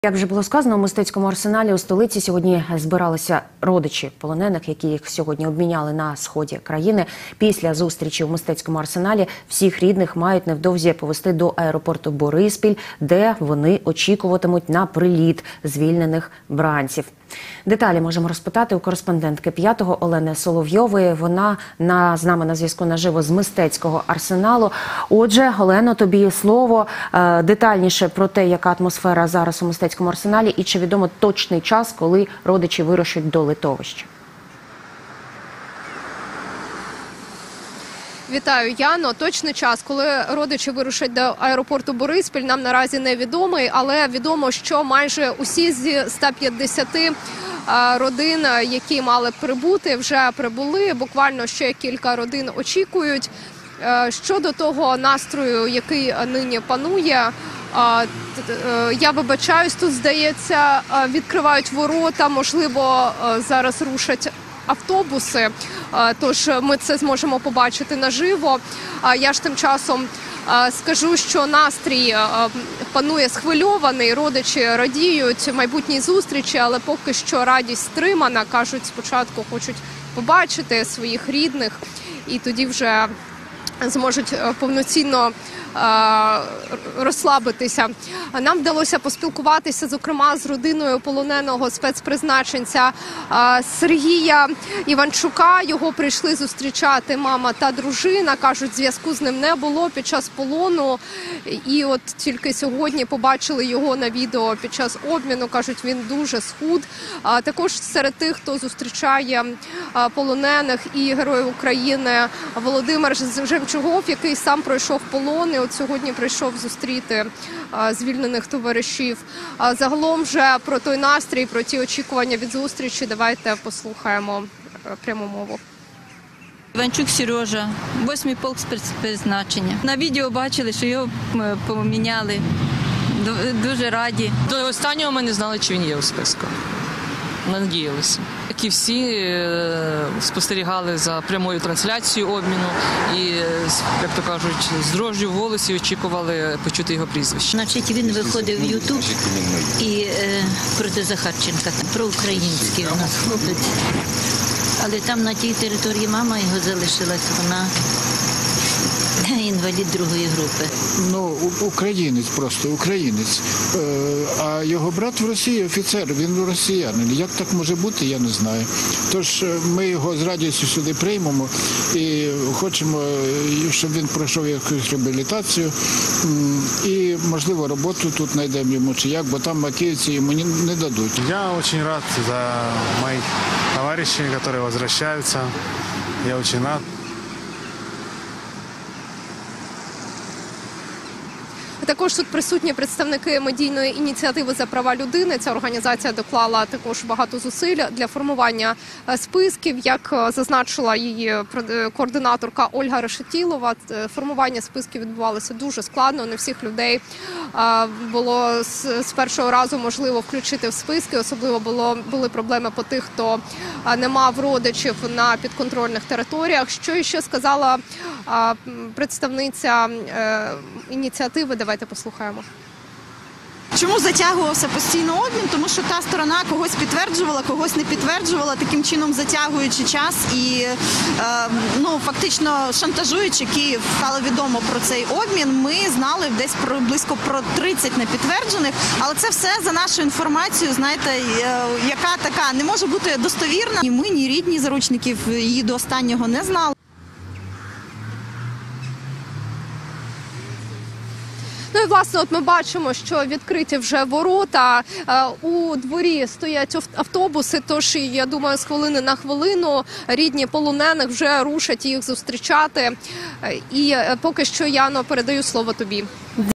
Великий обмін - відбувся: настрої родичів звільнених з полону українців / включення
У Мистецькому арсеналі у столиці збиралися родичі полонених, яких сьогодні обміняли на сході країни. Після зустрічі у Мистецькому арсеналі всіх рідних мають невдовзі повезти до аеропорту "Бориспіль", де вони очікуватимуть на приліт звільнених бранців.